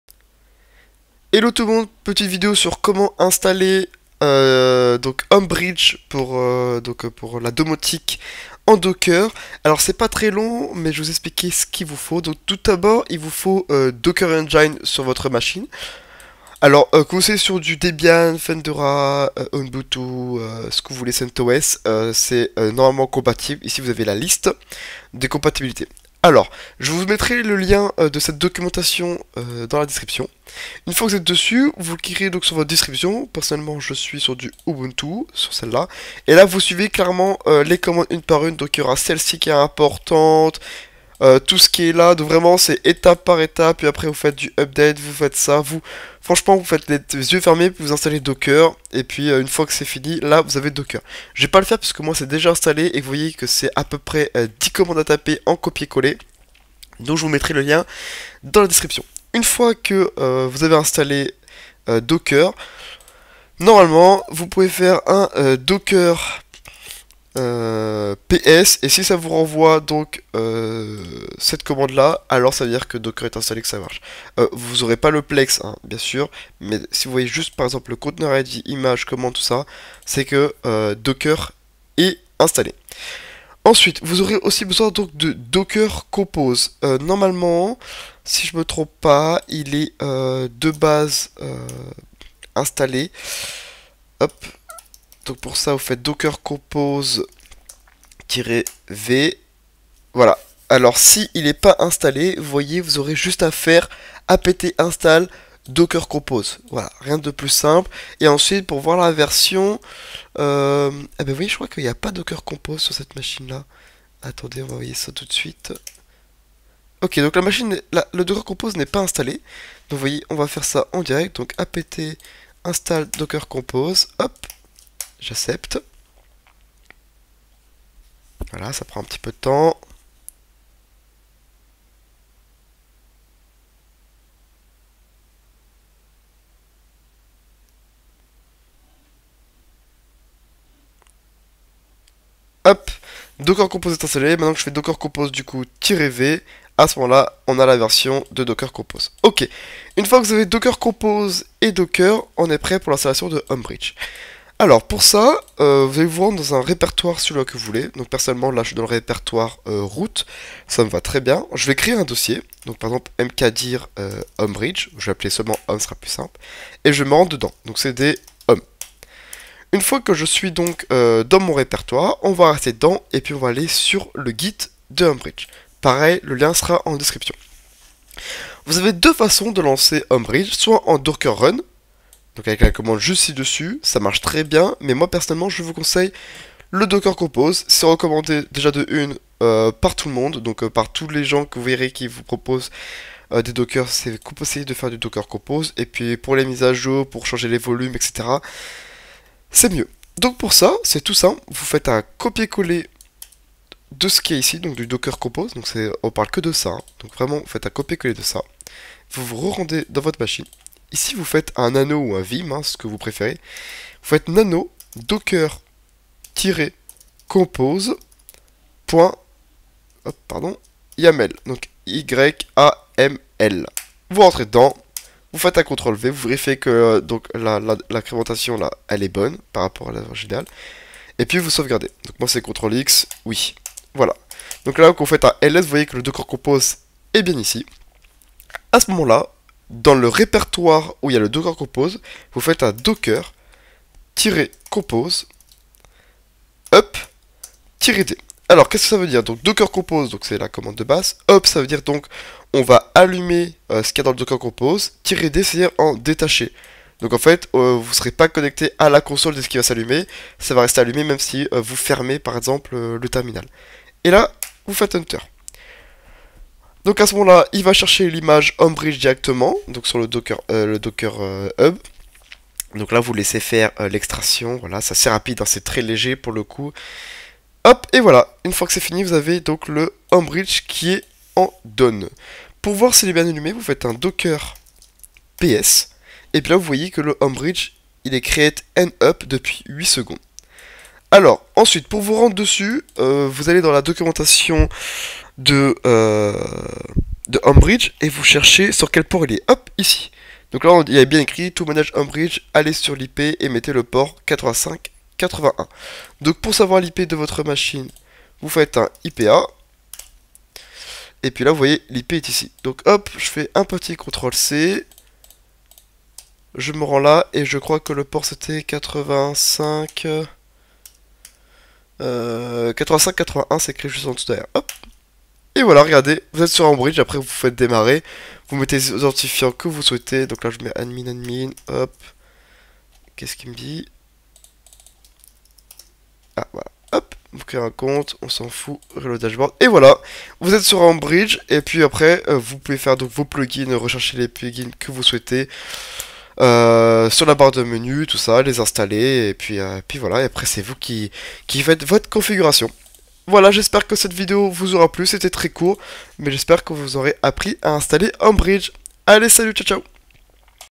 Comment installer HomeBridge en Docker !
Hello, ceci est la première vidéo d'une longue série sur comment installer des dockers. Aujourd'hui, nous allons nous attaquer à Homebridge, en rapport avec la vidéo sur la domotique. Si vous avez des questions, n'hésitez pas à les poser.